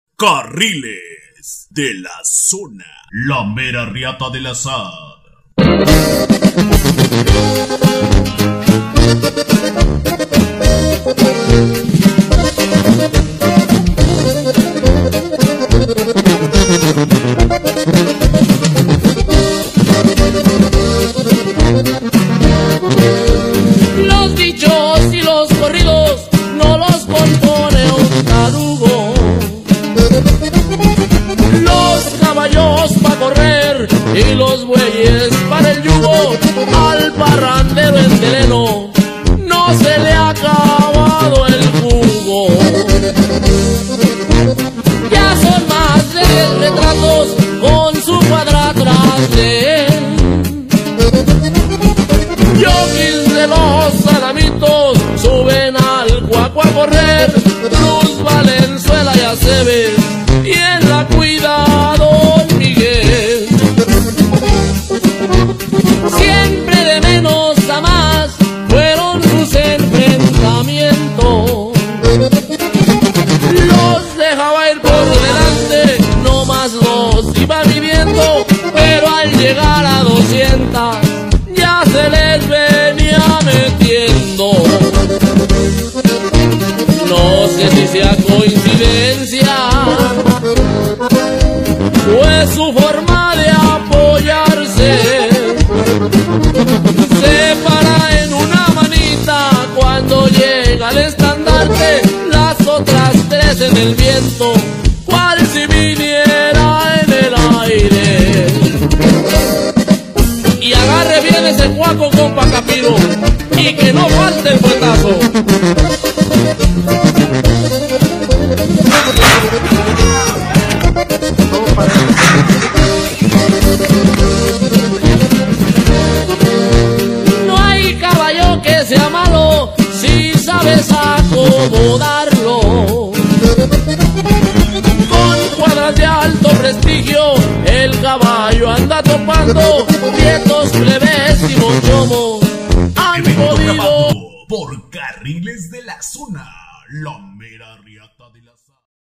5TO HIT ELIMINATORIO A 200 VARAS
Ganando su hit eliminatorio
EL SHERIFF de la cuadra jacatepec a el nariz tapada de la cuadra playa vicente
LOMA BONITA OAXACA
8/julio/16